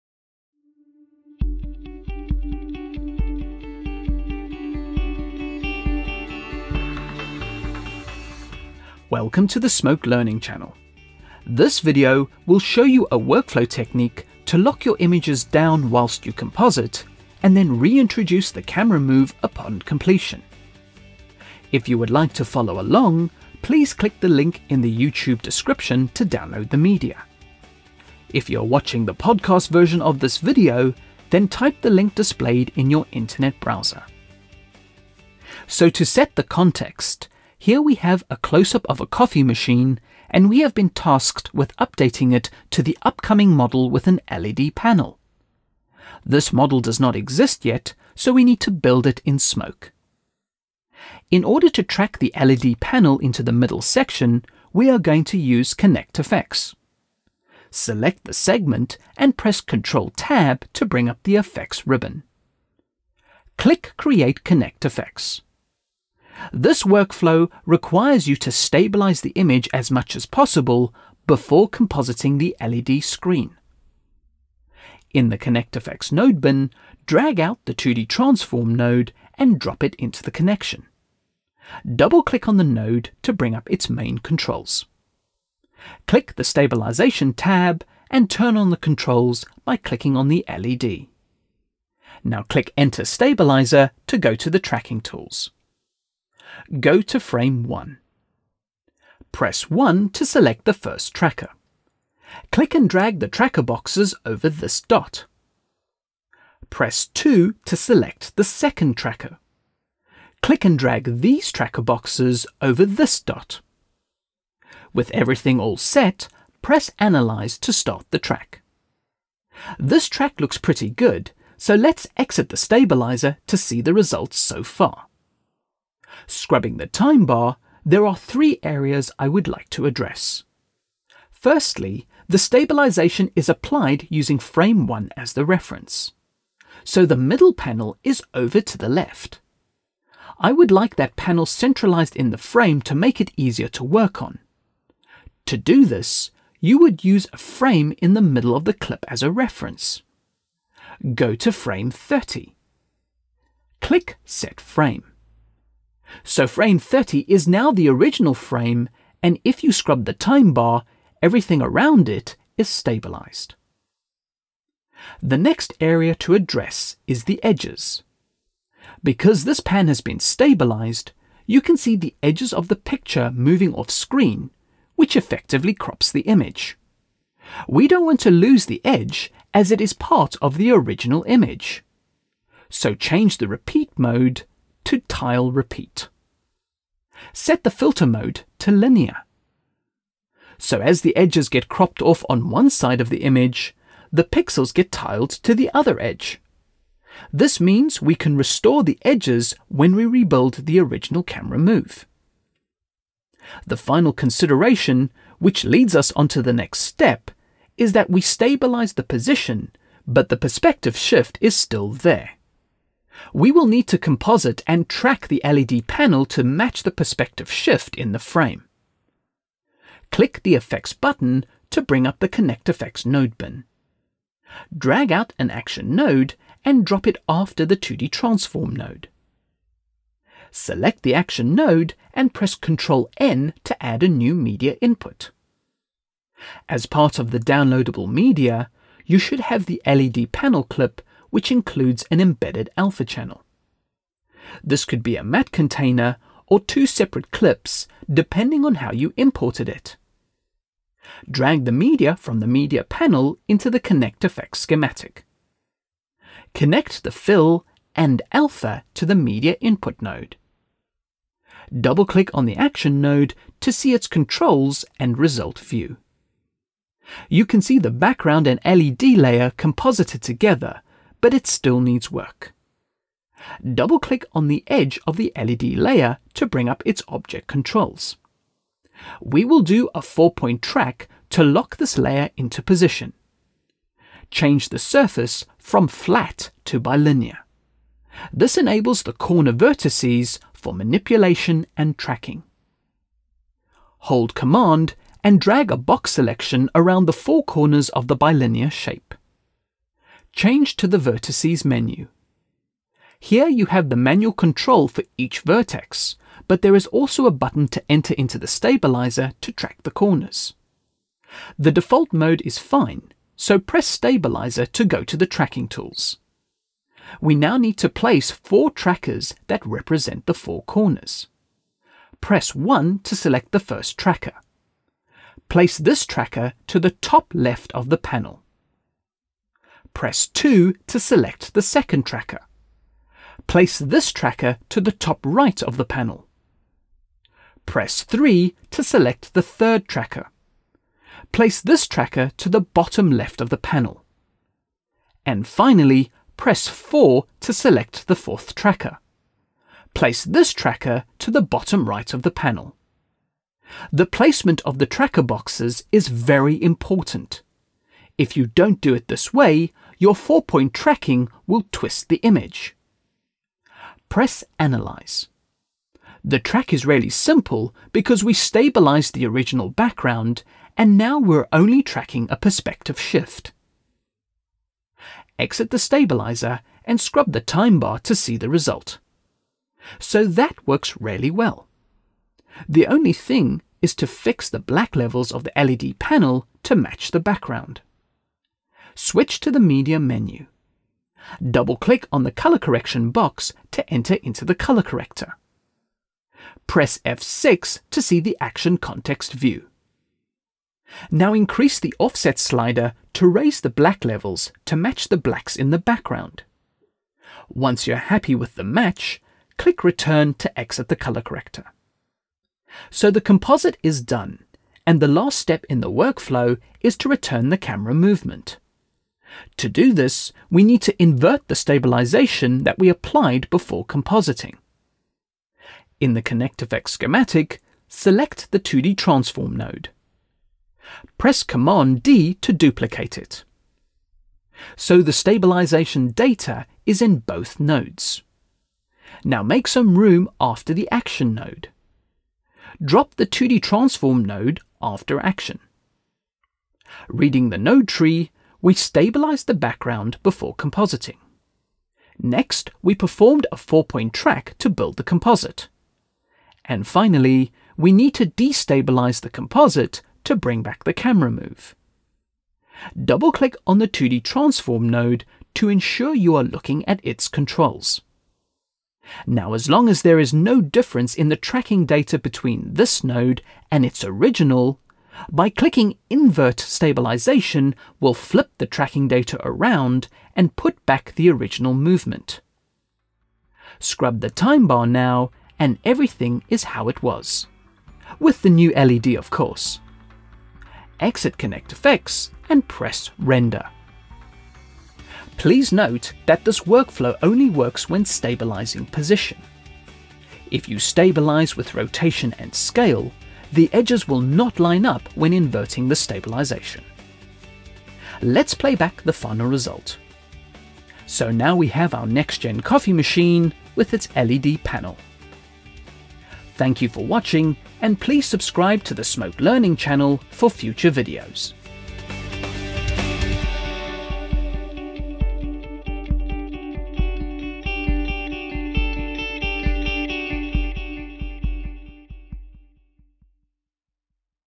Stabilization - Stabilize, Composite and Destabilize - Smoke 2013 Ext 1
Level: Intermediate
Recorded in: Autodesk Smoke 2013 Extension 1

This video series covers most of the workflows concerning stabilization.

This video shows you a compositing workflow to stabilize your image, composite an element with 4-point tracking and finally destabilize the image to add the camera back again.

For Information, Feedback and Comments:

Autodesk Area